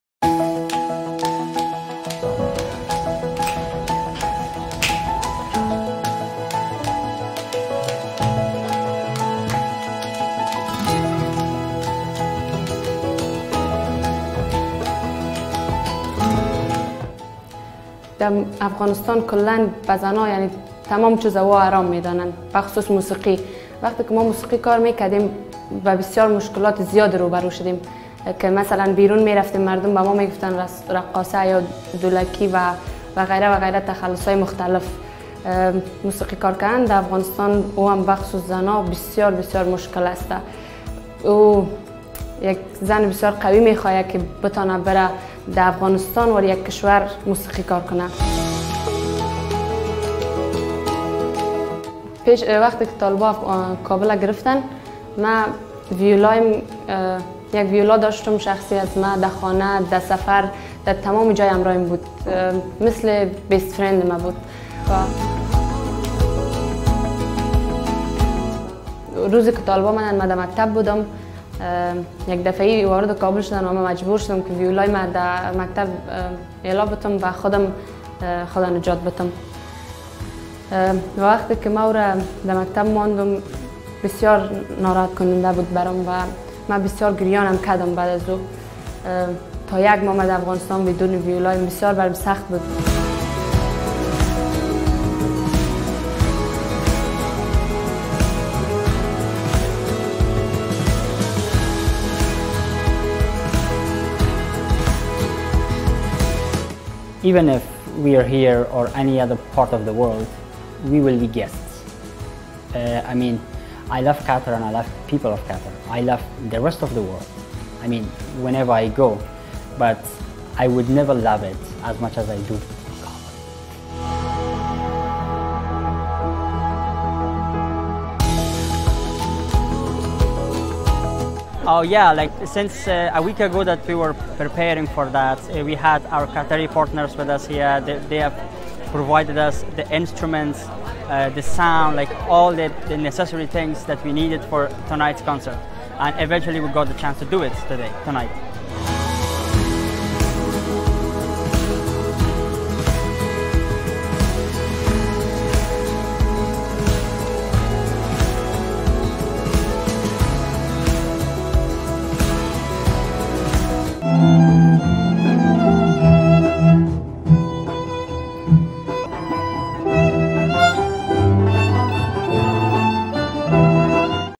الأوركسترا الأفغانية تلجأ إلى قطر !
مارزيا ، عازفة كمان أفغانية ، تروي اللحظة التي اضطرت فيها إلى التخلي عن أعز أصدقائها بعد سيطرت طالبان على كابول !